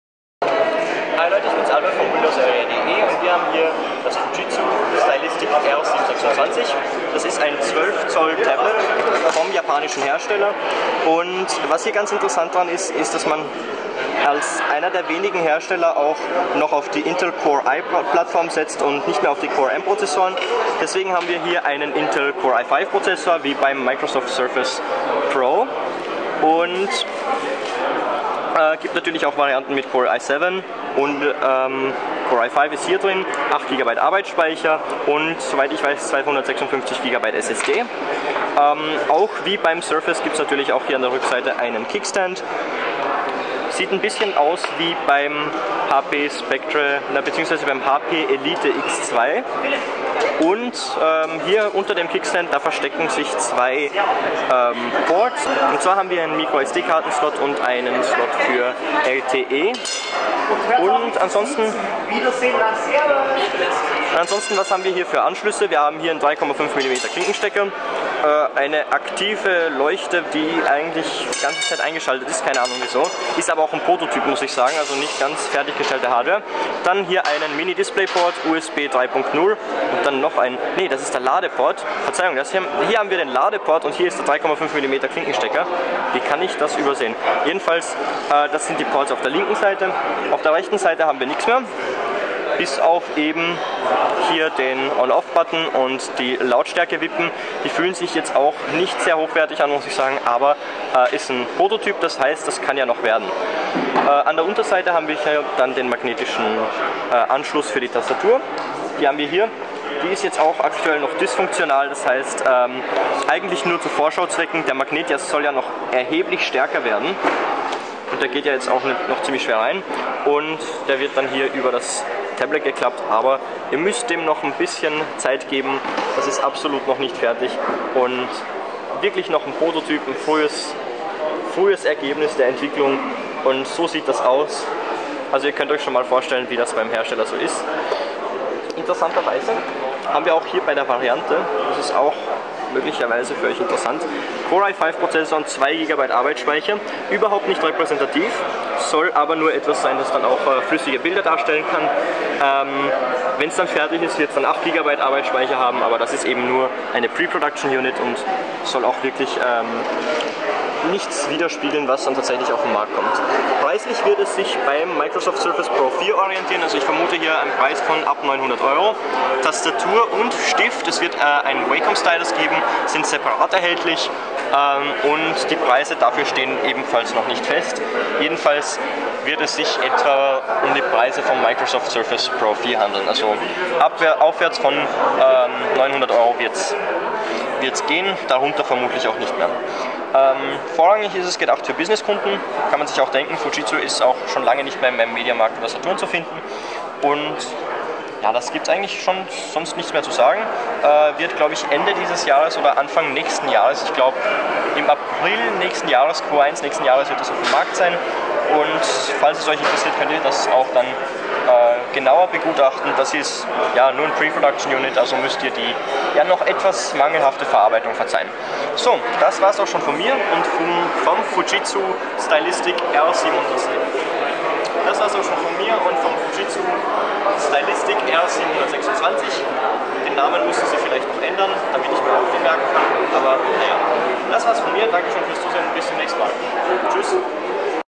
Fujitsu Stylistic R726 Hands-On und erster Eindruck (Prototyp!)
Das Hands-On haben wir noch 2015 gemacht, das Gerät kommt in Kürze ganz ohne Prototyp-Status auf den Markt. Die Audioqualität ist leider etwas schwach, nächstes Mal haben wir ein Mikrofon mit.

WindowsArea.de ist ein deutschsprachiges Windows-Magazin, das euch über alle relevanten News aus dem Microsoft Ökosystem auf dem Laufenden hält.